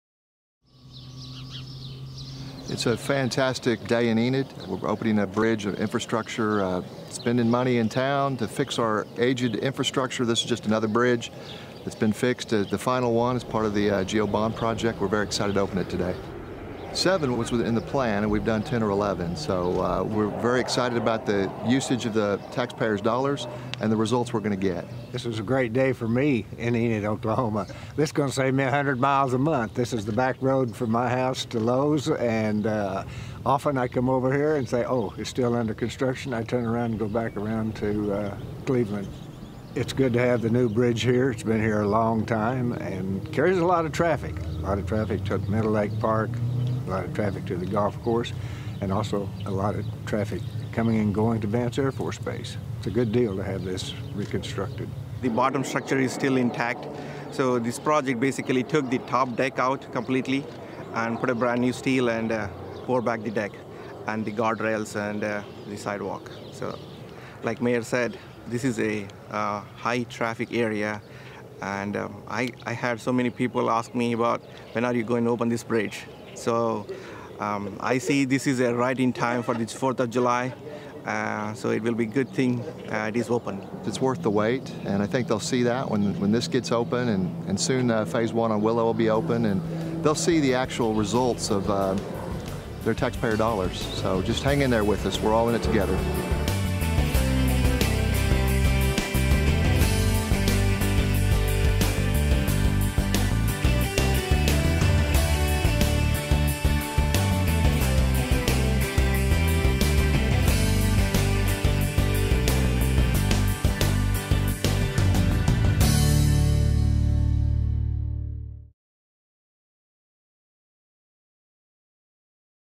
Rupe Bridge Re-Opening - Enid, Oklahoma - June 26, 2017
City officials announced today the reopening of the Rupe Avenue bridge.   The Rupe bridge project was completed in 180 days and included the rehab of the bridge deck, the diaphragm between the beams of the structure, and new guardrails.

The Rupe bridge project was part of the June 2008 $7 million general obligation bonds project impacting a total of 12 bridges exceeding the goal of seven bridges.